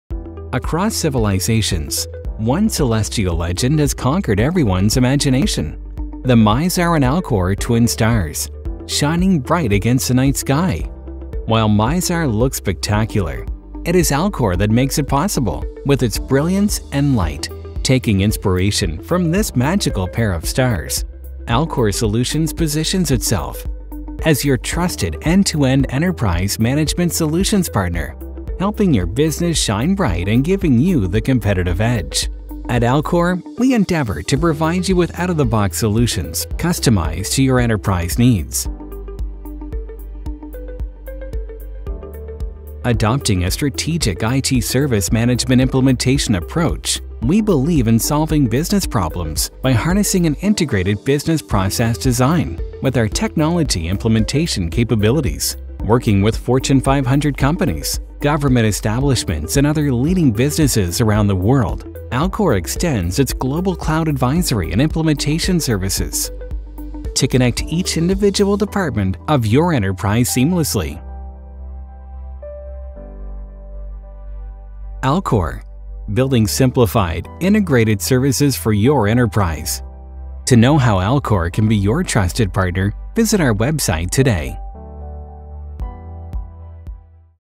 The Genesis of Alcor- Building Simplified, Integrated Services For Your Enterprise
Alcor is a global cloud advisory and implementation services company serving Fortune 500, Government establishments, and other leading organizations in multiple industry verticals across the Americas, Canada and India.  Alcor is a ServiceNow Silver partner and also partners to Mulesoft, Salesforce, Tanium, Microsoft and Bomgar.  They advise leading businesses on cloud platforms, architecture, enterprise service management and integrating IT service delivery. They also provide business process consulting to capture, re-engineer and improve processes that can easily be automated to deliver real value. The Alcor consulting team is derived from a combination of experts in Business strategy, Cloud Technology and Organizational Change Management.